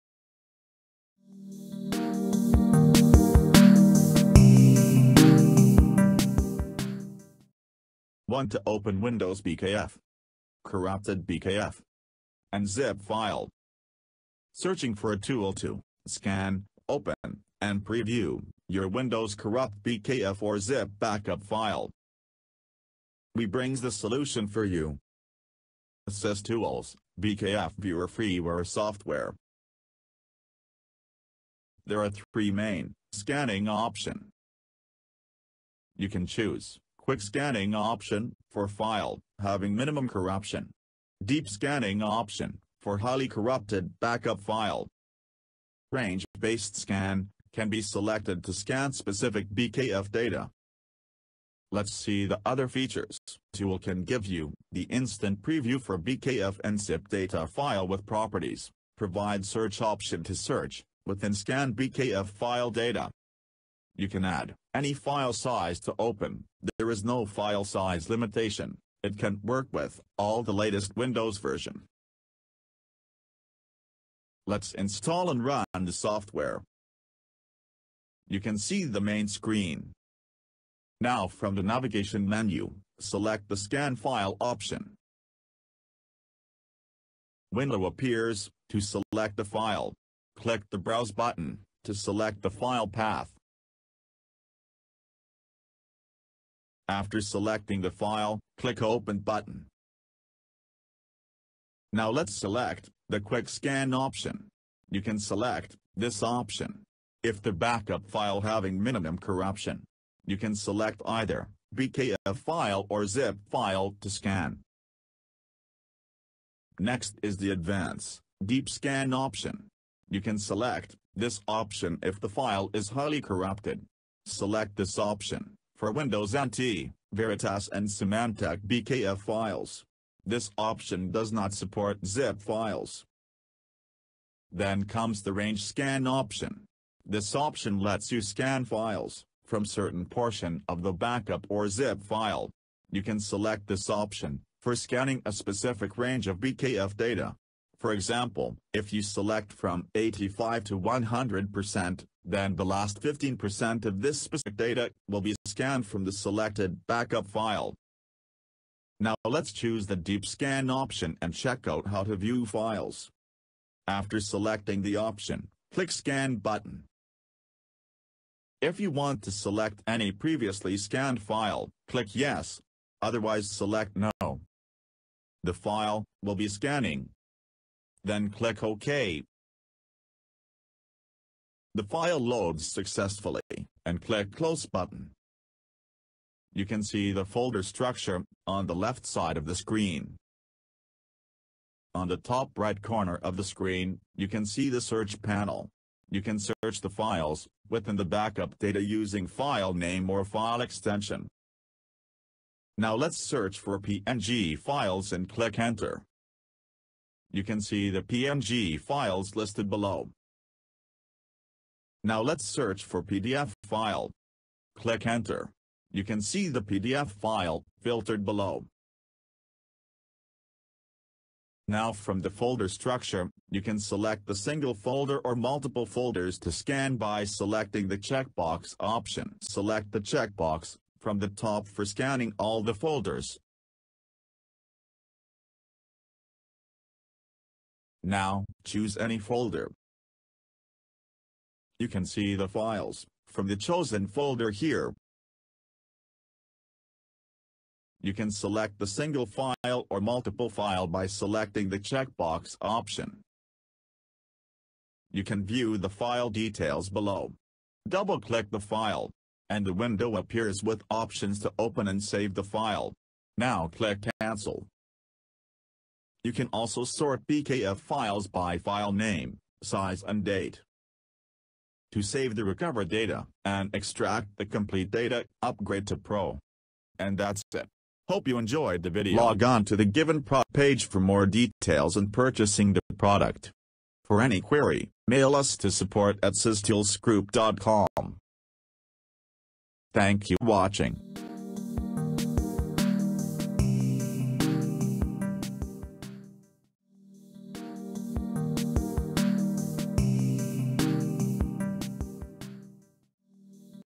SysTools BKF Viewer FREEWARE [Official] - How to Open a .BKF File in Windows
Free BKF Viewer Utility which assists you to view, open and read BKF files.

Learn How to Open a.BKF file in Windows and access file data with this freeware.